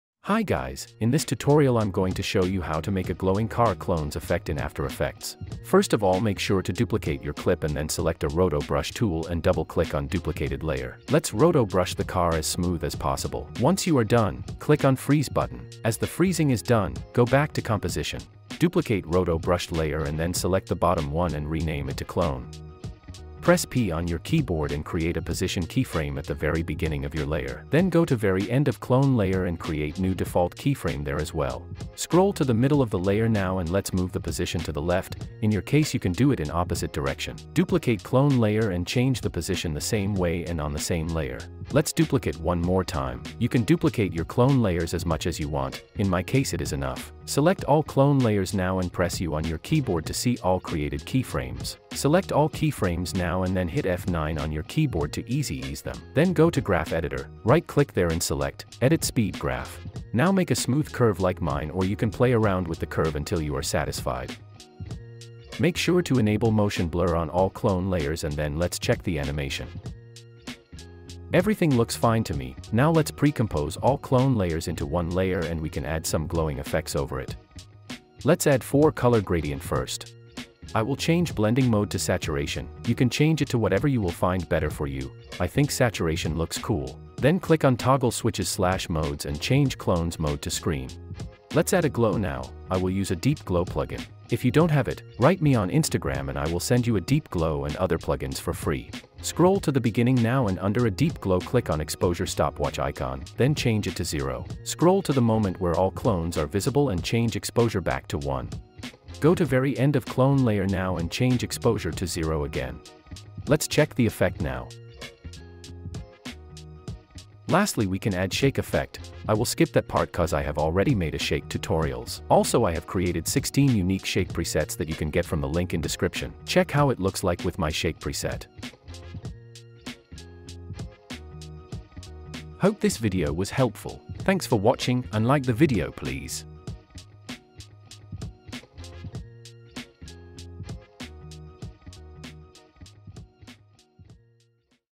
Glowing Clone Effect in After Effects
In this tutorial I will show you how to make a glowing clone effect in Adobe After Effects

CPU: i5-12400F
GPU: GeForce RTX3050 VENTUS 8GB
RAM: Kingston FURY Beast 32GB